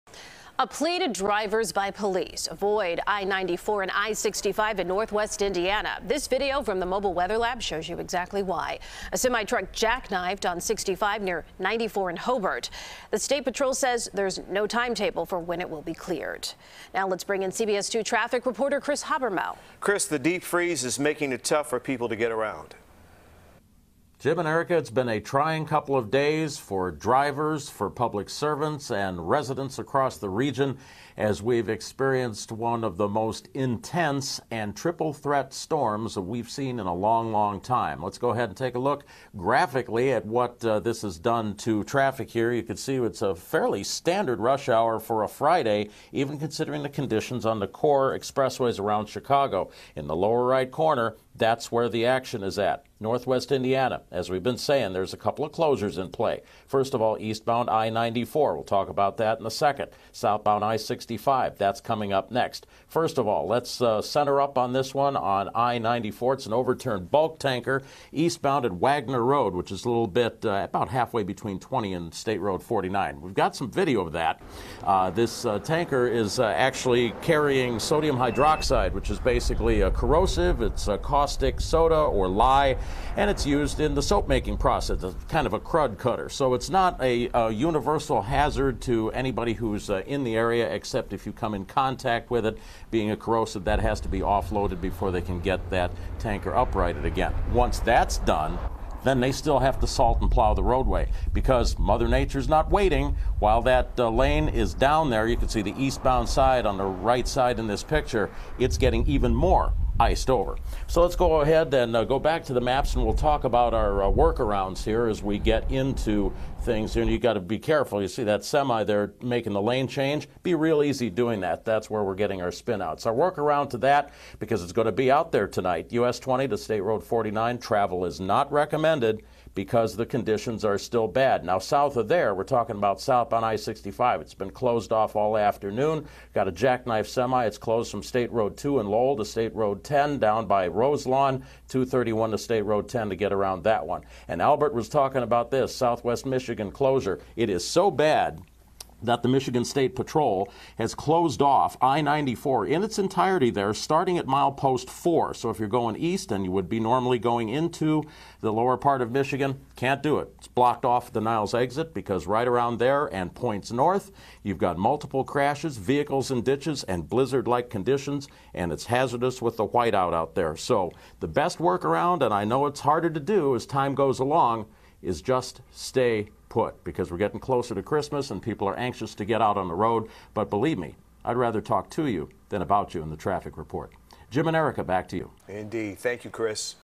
Crashes shut down interstates in Northwest Indiana
A tanker truck shut down a stretch of eastbound I-94, while a jackknifed semi shut down part of I-65. Kris Habermehl has a breakdown of the treacherous conditions.